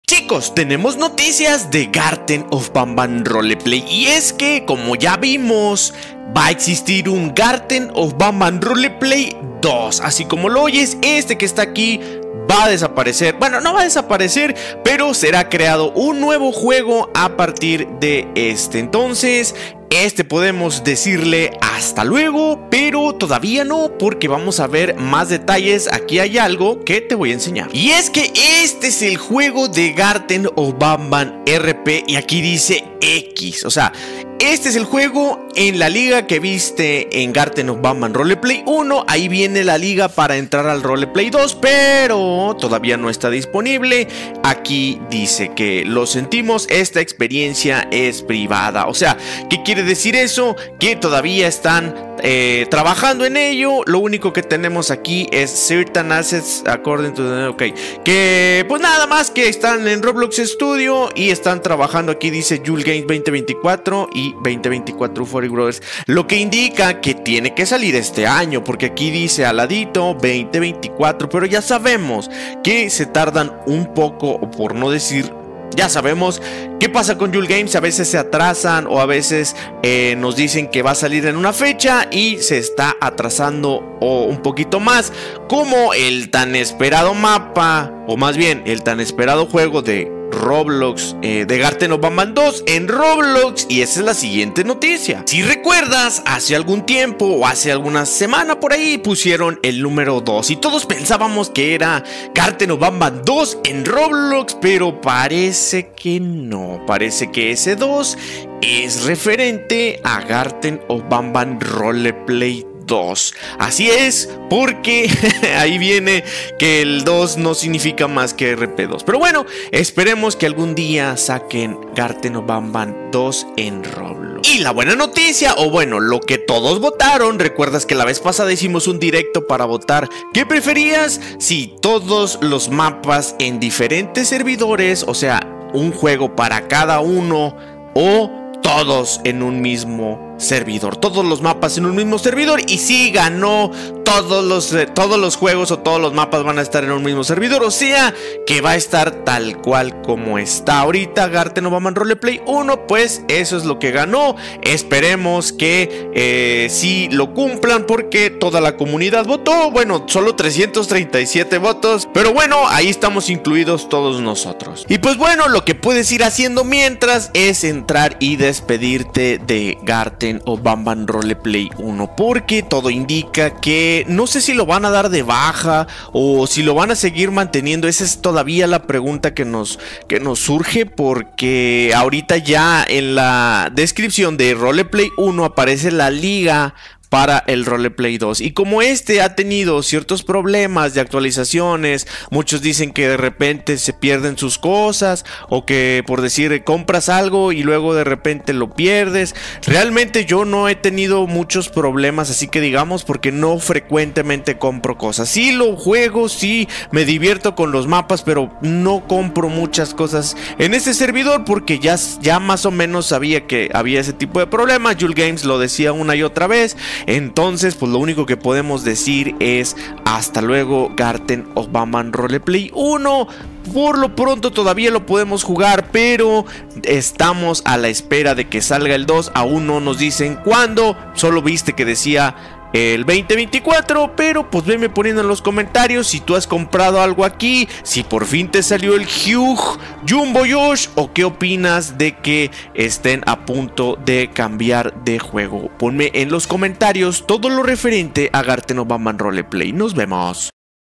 NOTICIAS DE GARTEN OF BANBAN ROLEPLAY 2 TE CUENTO TODOS LOS DETALLES
CHICOS POR FIN TENDREMOS UNA NUEVA ACTUALIZACION DE GARTEN OF BANBAN ROLEPLAY! POR FIN ALGO NUEVO!

En este video analiaremos un juego de GARTEN OF BANBAN ROLEPLAY sobre GARTEN OF BANBAN 8